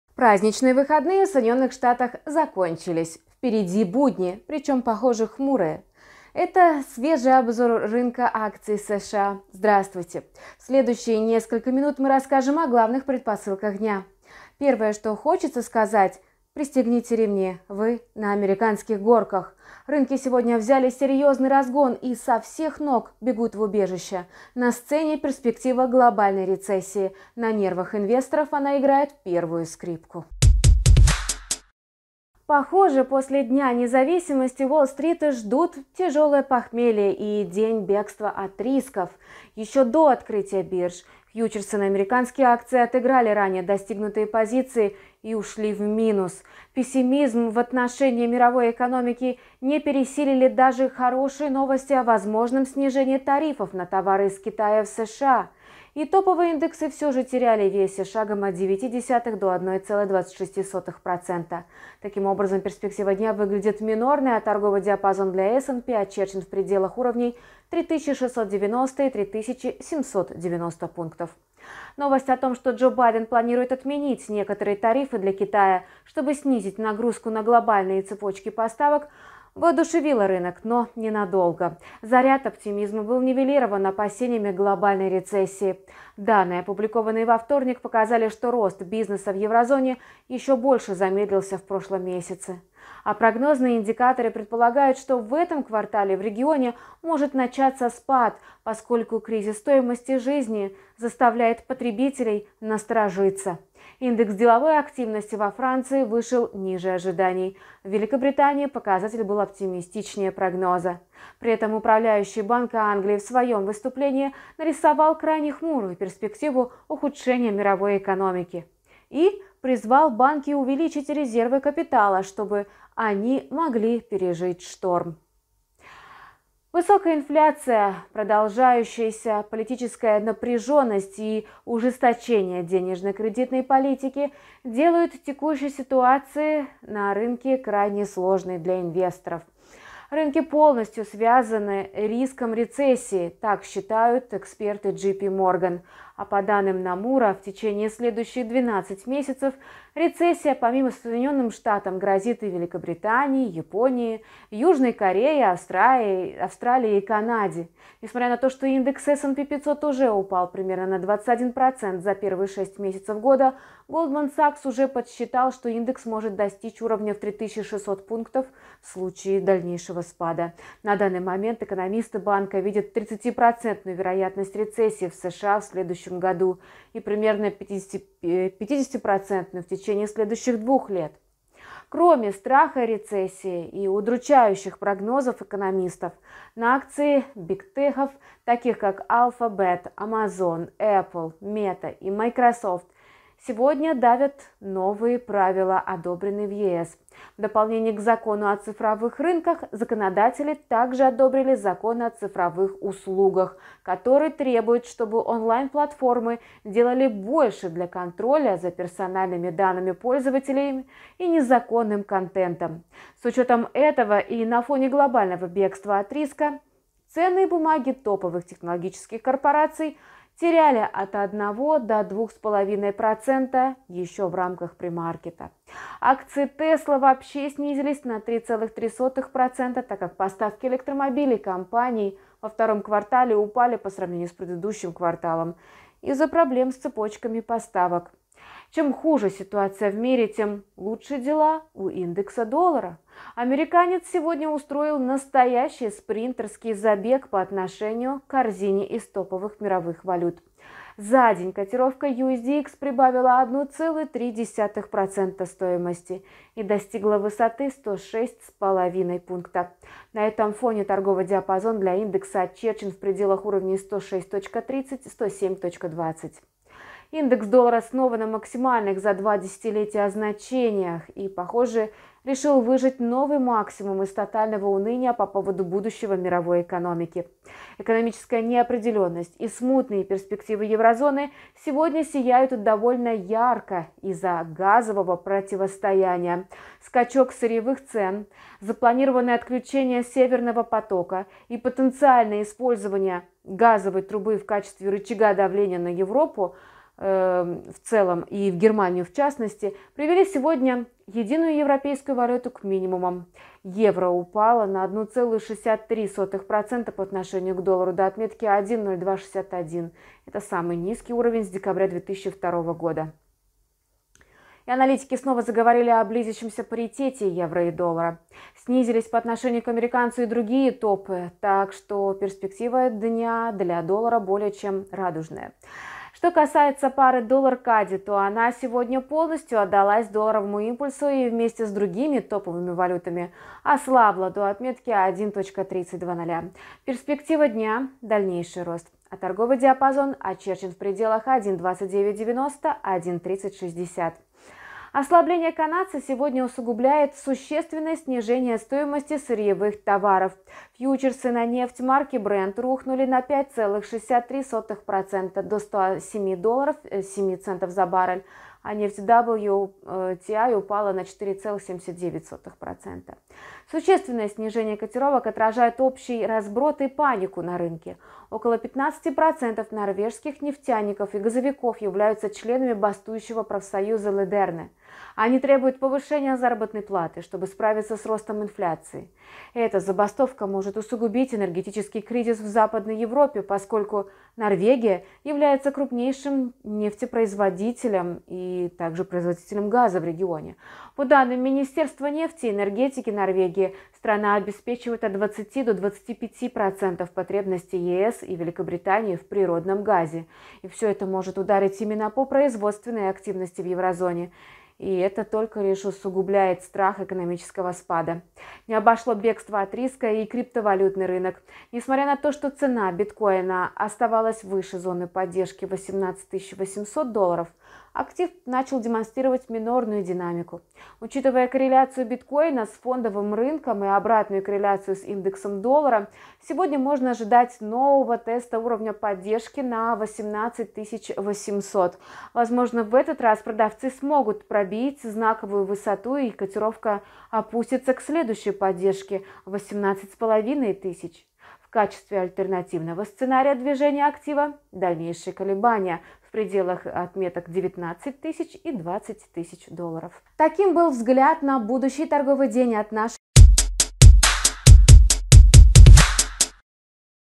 05.07.2022: В бегстве от риска рынок перешел с марафона на спринт (S&P500, USD, CAD, Bitcoin)
Праздничные выходные в США закончились. Впереди будни, причем, похоже, хмурые. Это свежий обзор рынка США, здравствуйте. В следующие несколько минут мы расскажем о главных предпосылках дня. Первое, что хочется сказать: пристегните ремни, вы на американских горках. Рынки сегодня взяли серьезный разгон и со всех ног бегут в убежища. На сцене – перспектива глобальной рецессии. На нервах инвесторов она играет первую скрипку.
Похоже, после Дня независимости Уолл-Стрит ждут тяжелое похмелье и день бегства от рисков. Еще до открытия бирж фьючерсы на американские акции отыграли ранее достигнутые позиции и упали в минус. Пессимизм в отношении мировой экономики не переселили даже хорошие новости о возможном снижении тарифов на товары из Китая в США, и топовые индексы теряли в весе шагом 0,9–1,26%. Таким образом, перспектива дня выглядит минорной, а торговый диапазон для S&P 500 очерчен в пределах 3 690 – 3 790.
Новость о том, что Джо Байден планирует отменить некоторые тарифы для Китая, чтобы снизить нагрузку на глобальные цепочки поставок, воодушевила рынок, но ненадолго. Заряд оптимизма был нивелирован опасениями глобальной рецессии. Данные, опубликованные во вторник, показали, что рост бизнеса в еврозоне еще больше замедлился в прошлом месяце, а прогнозные индикаторы предполагают, что в этом квартале в регионе может начаться спад, поскольку кризис стоимости жизни заставляет потребителей насторожиться. Индекс деловой активности во Франции вышел ниже ожиданий, в Великобритании показатель был оптимистичнее прогноза. При этом управляющий Банка Англии в своем выступлении нарисовал хмурую перспективу ухудшения мировой экономики и призвал банки увеличить резервы капитала, чтобы они могли пережить шторм.
Высокая инфляция, продолжающаяся политическая напряженность и ужесточение денежно-кредитной политики делают текущую ситуацию сложной для инвесторов. Рынки полностью связаны риском рецессии, считают в JPMorgan. А по данным Nomura, в течение следующих 12 месяцев рецессия, помимо США, грозит Великобритании, Японии, Южной Корее, Австралии и Канаде. Несмотря на то что индекс S&P 500 уже упал примерно на 21% за первые шесть месяцев года, Goldman Sachs уже подсчитал, что индекс может достичь 3 600 пунктов в случае спада. На данный момент экономисты банка видят 30-процентную вероятность рецессии в США в следующем году и примерно 50-процентную – в течение следующих двух лет.
Кроме страха рецессии и удручающих прогнозов экономистов, на акции бигтехов, таких как Alphabet, Amazon, Apple, Meta и Microsoft, сегодня давят новые правила, одобренные в ЕС. В дополнение к правилам, известным как Закон о цифровых рынках (DMA), законодатели также одобрили Закон о цифровых услугах (DSA), который требует, чтобы онлайн-платформы делали больше для контроля за персональными данными пользователей и незаконным контентом. С учетом этого и на фоне глобального бегства от риска ценные бумаги топовых технологичных корпораций теряли 1–2,46% в рамках премаркета. Акции Tesla снизились на 3,03%, так как поставки электромобилей компании во втором квартале упали по сравнению с предыдущим кварталом из-за проблем с цепочками поставок.
Чем хуже ситуация в мире, тем лучше дела у индекса доллара. «Американец» сегодня устроил настоящий спринтерский забег по отношению к корзине из топовых мировых валют. За день USDX прибавил 1,3% стоимости и достиг высоты 106,5. На этом фоне торговый диапазон для индекса очерчен в пределах 106,3–107,2.
Индекс доллара снова на максимальных за два десятилетия значениях и, похоже, решил выжать новый максимум из тотального уныния по поводу будущего мировой экономики. Экономическая неопределенность и смутные перспективы еврозоны сегодня сияют довольно ярко из-за газового противостояния. Скачок сырьевых цен, запланированное отключение «Северного потока» и потенциальное использование газовой трубы в качестве рычага давления на Европу в целом и Германию в частности привели сегодня евро к минимумам. Единая европейская валюта упала на 1,63% по отношению к доллару, до 1,0261 доллара, самого низкого с декабря 2002 года уровня. Аналитики снова заговорили о близящемся паритете евро и доллара, снизились по отношению к «американцу» и другие топы, так что перспектива дня для доллара более чем радужная.